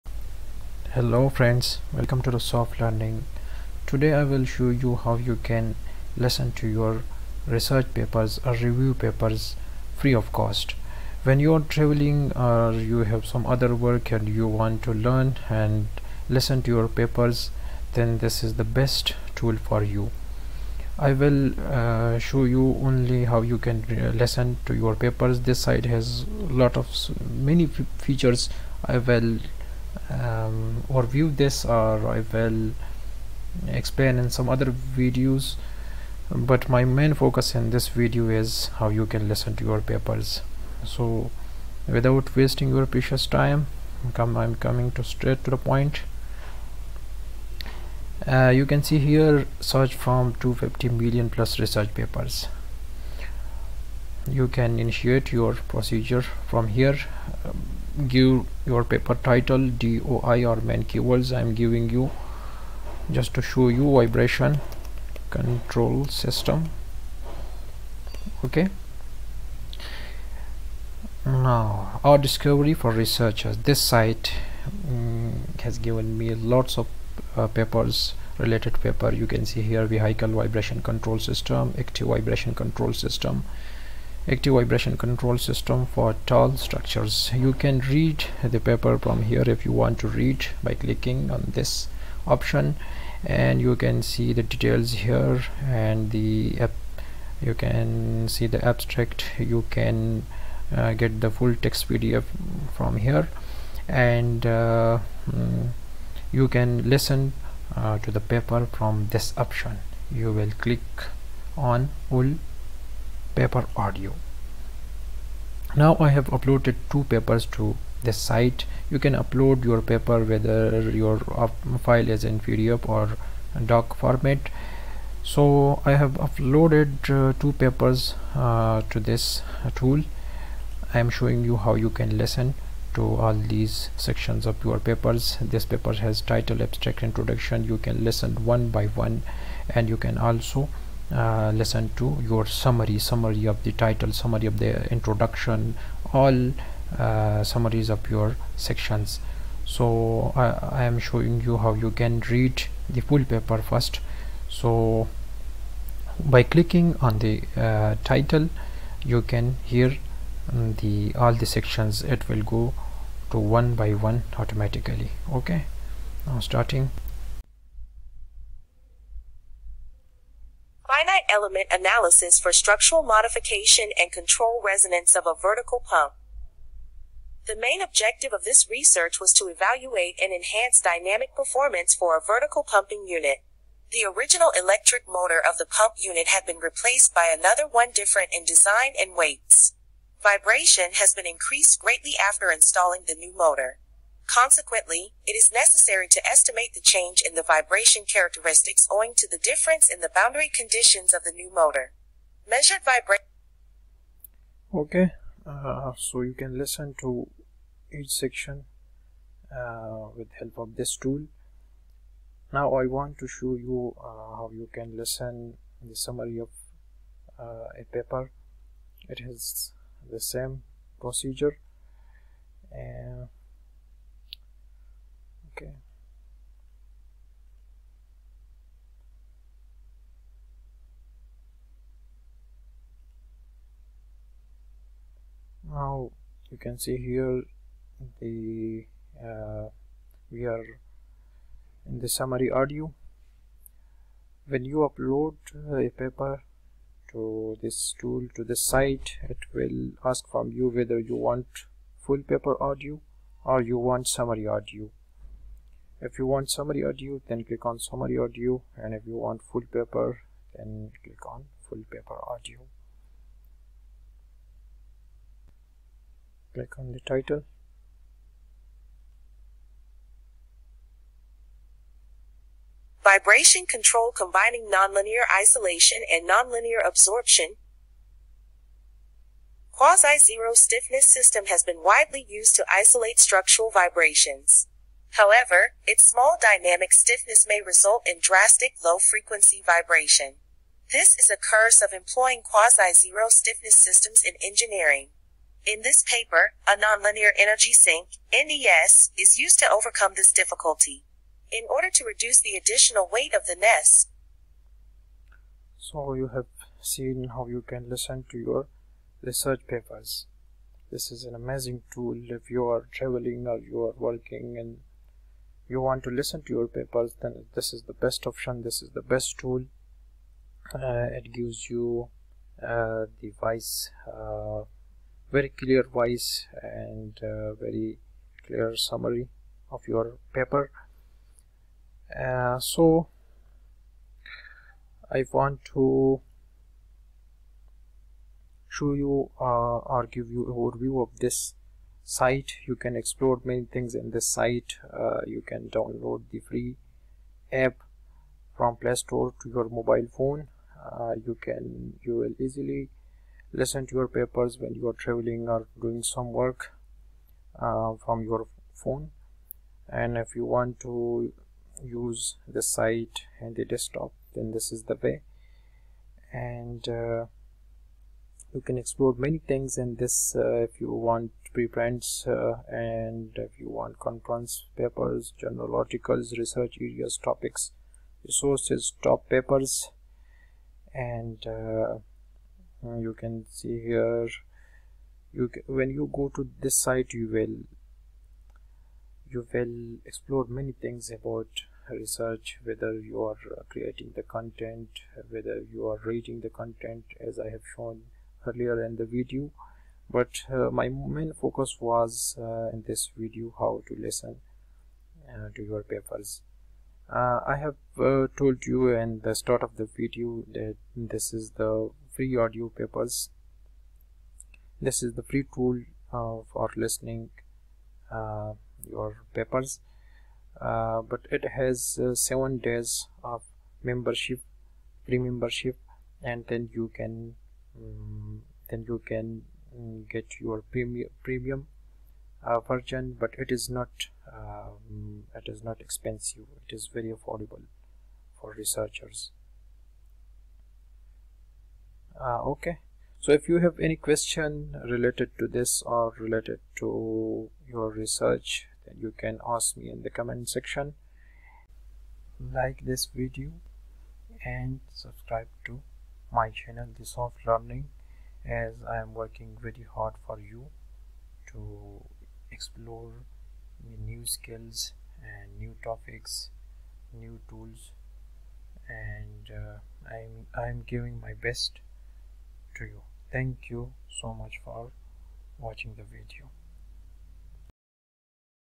Audio Research Papers 🎧📄| Listen when you can't read!
Now, it's easy to listen to your complete research papers. Understanding your research papers is crucial for your academic success. In your master's or PhD, you have a lot of work to do. Listening to your research papers or review papers will ease your burden of learning and understanding. It will also save you time!! After watching the video, it is easy to read, listen and publish.

A tool used in this video is R Discovery for researchers. Enjoy the easiest procedure ever.